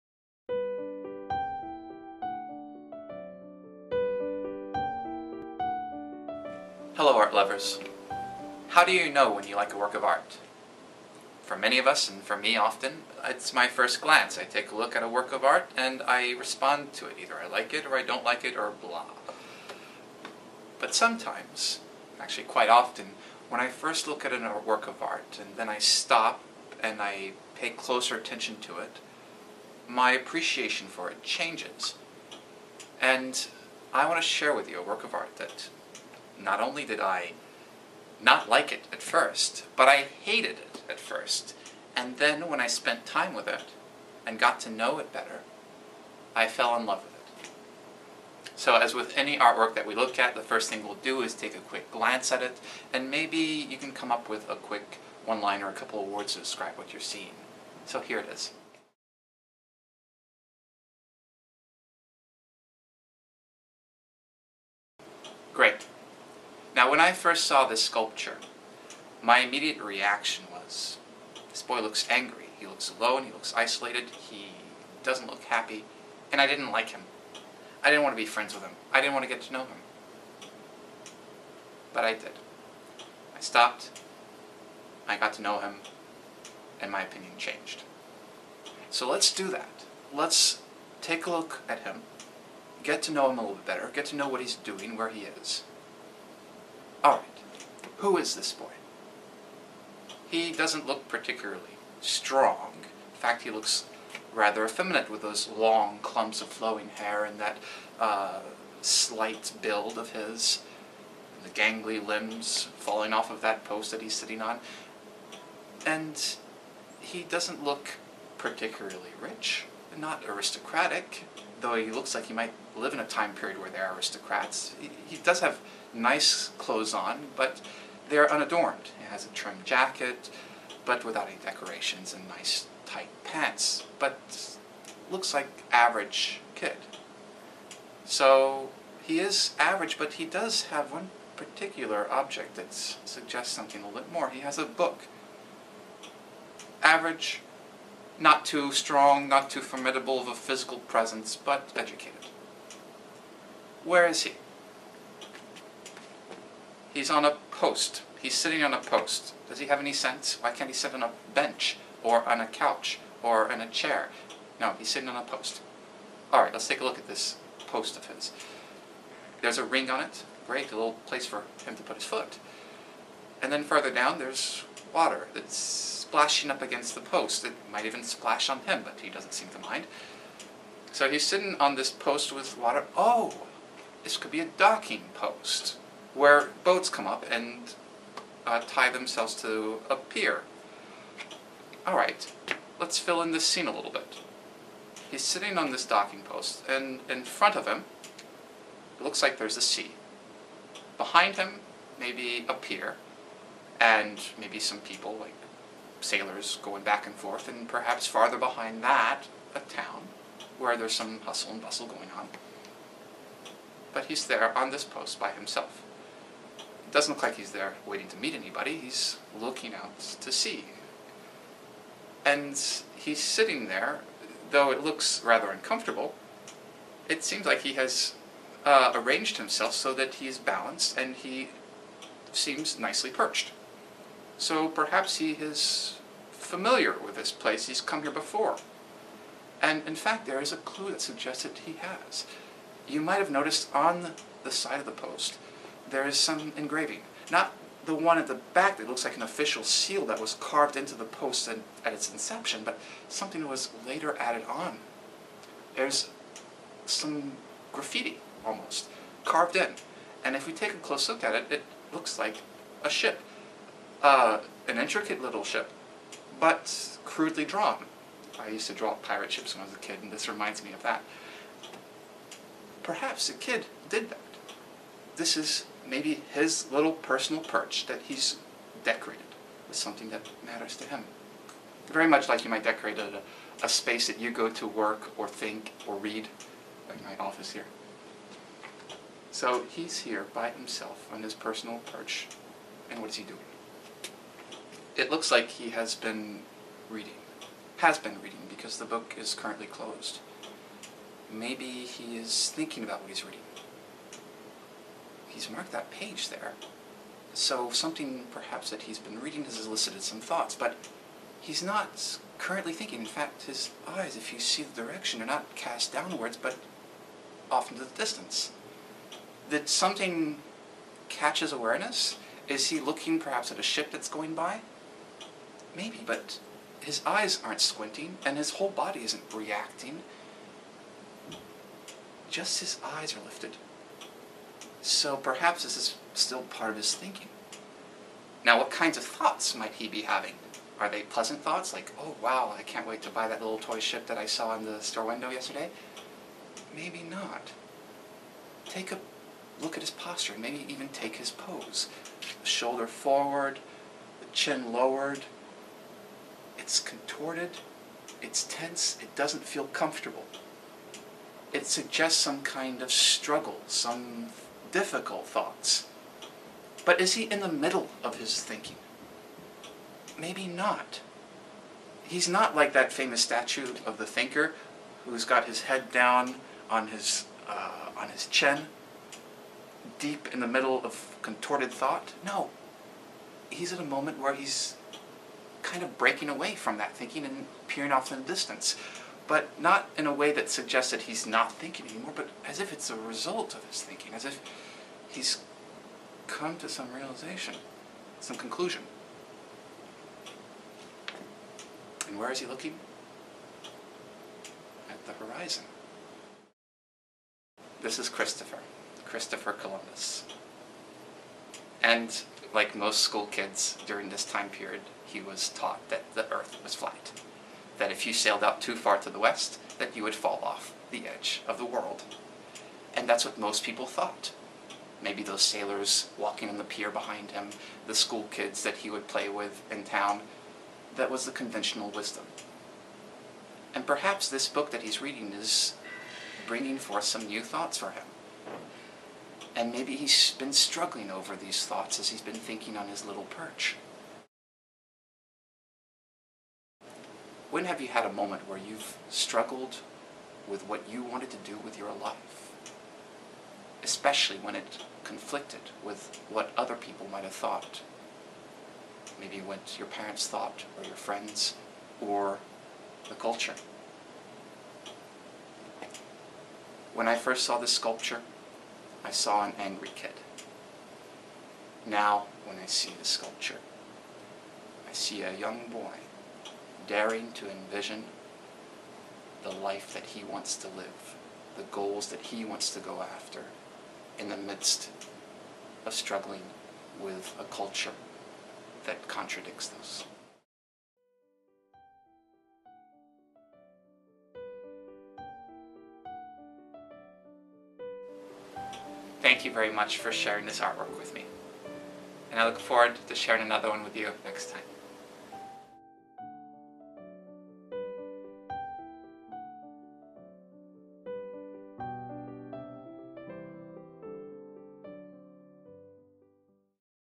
Bringing an Artwork to Life: A Young Rebel
Immerse yourself in the world of a beautiful work of art, experience the drama of the "story," and personally connect to the characters and their moment.